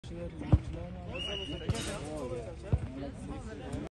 Talabalar_ligasi
Talabalar futbol ligasining K va L guruhida 3-tur o'yinlari bo'lib o'tdi.